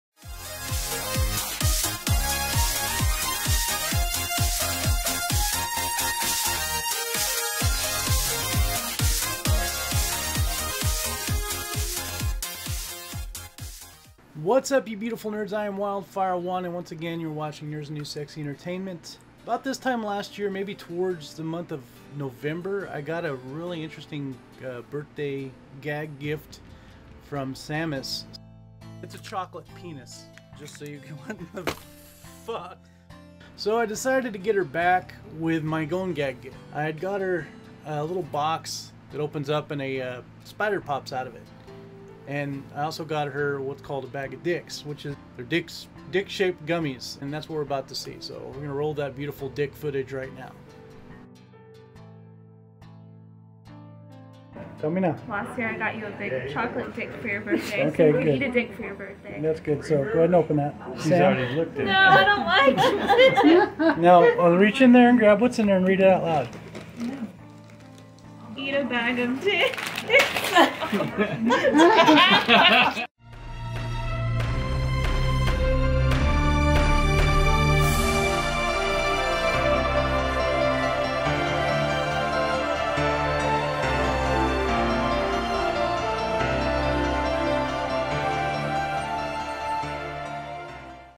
Sammus Prank Gift payback!
Around this time last year Sammus bought Wyldfyre1 a mystery gift that turned out to be a big chocolate weener! So Wyld decided to get her back this year for her own birthday and this is how he did it!

0:00 opening
0:13 Video beginning